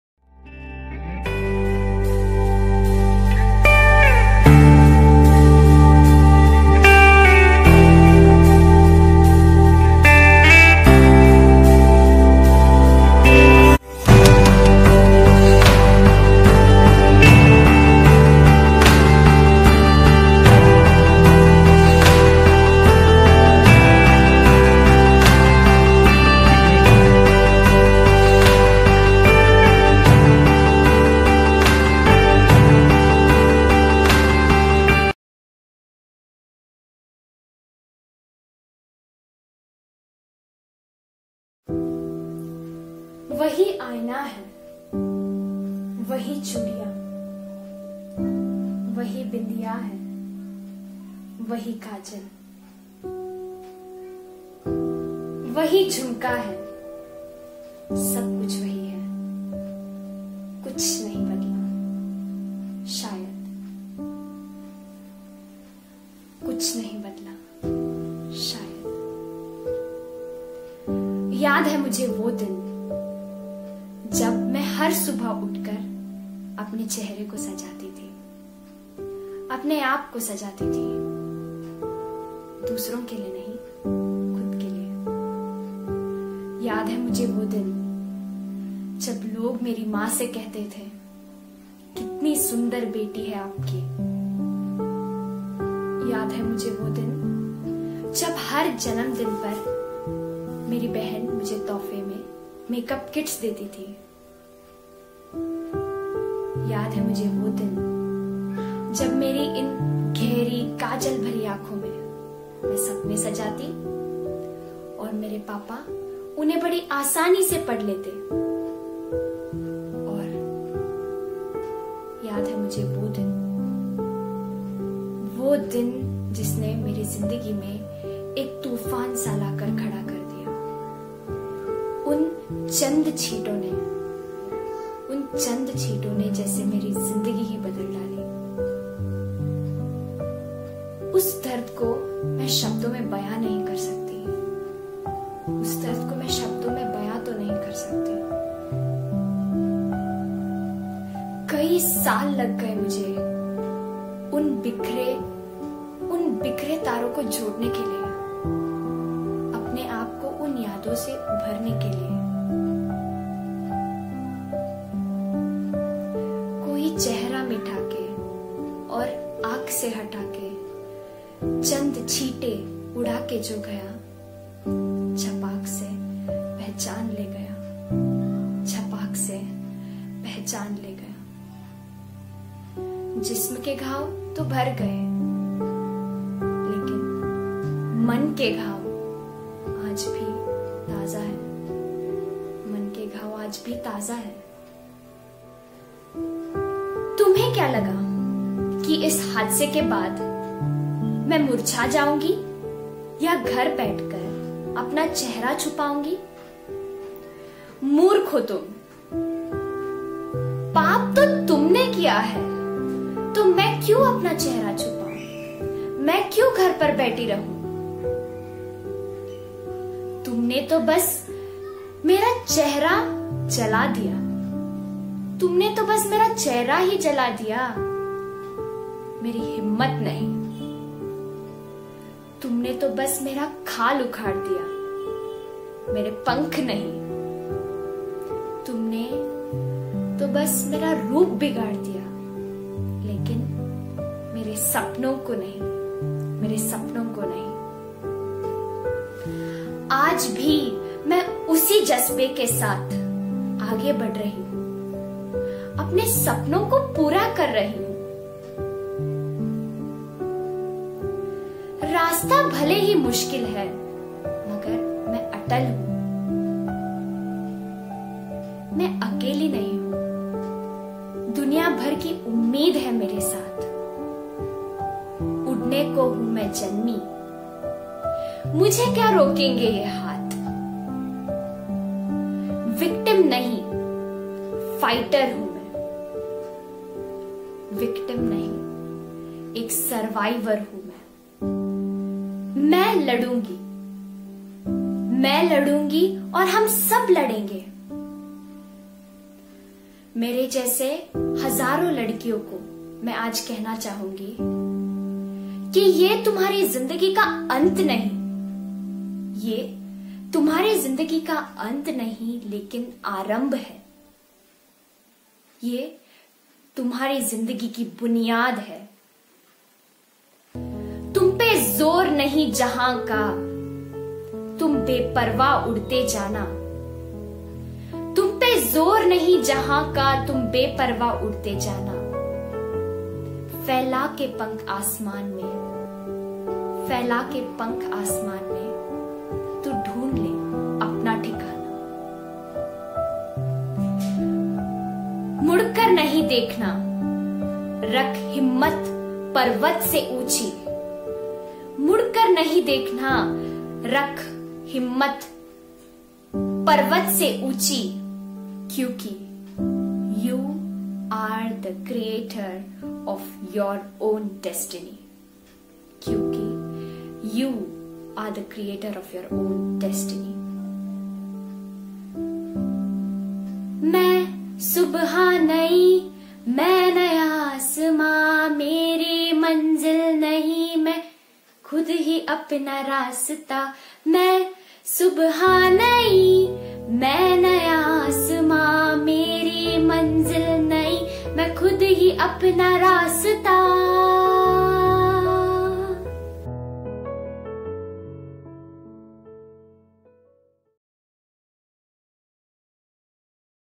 Monoact On Acid Attack Survivors| AADHAR|NSS Fergusson College
Aadhar group conducted Monoact activity on acid attack survivors in the month of December.